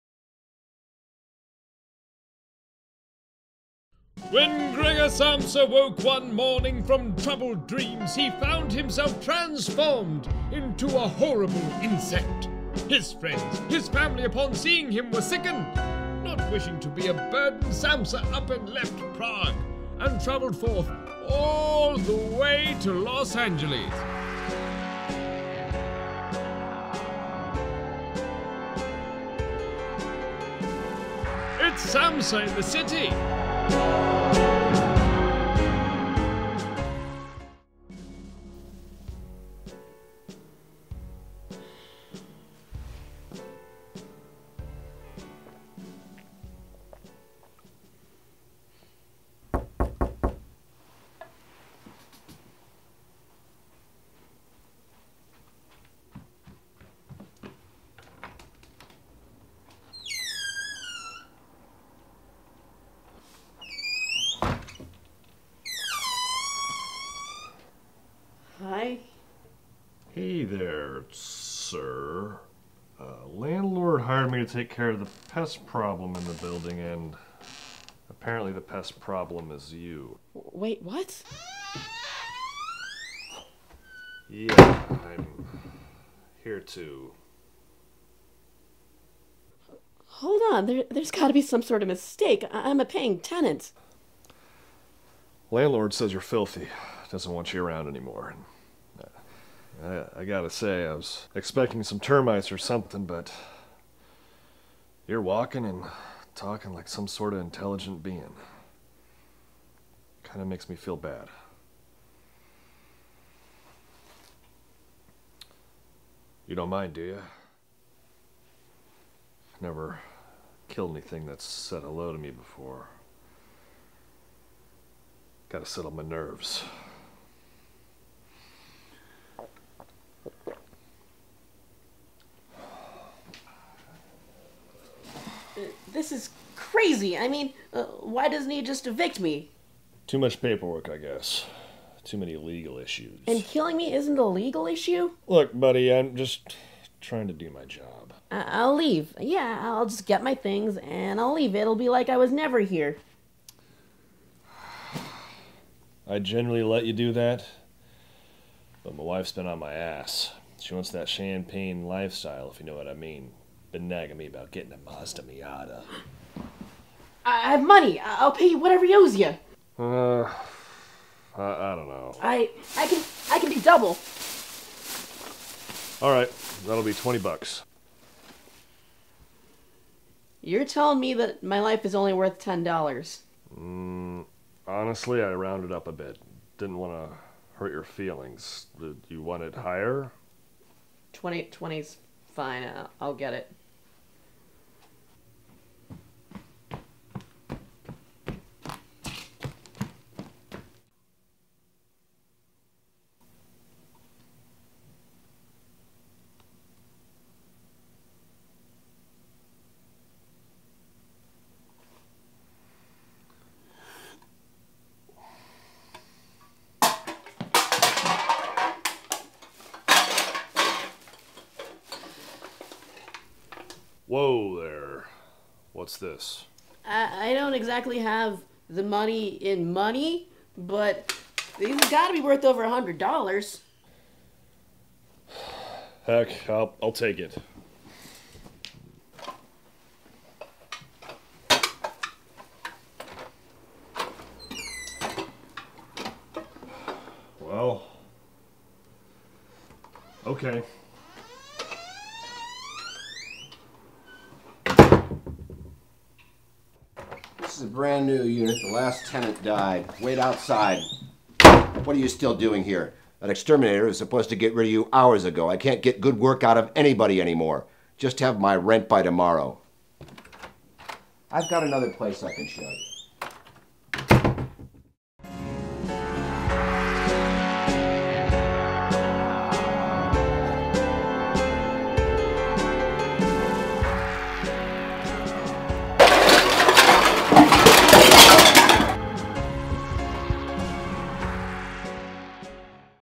Samsa in the City Ep 1
Samsa has trouble with an exterminator and his landlord
Written By: Michael Chau
Director: Sean George
Starring: Michael Chau
Featuring: Ryan Miller, Rama Vallury, & Ron West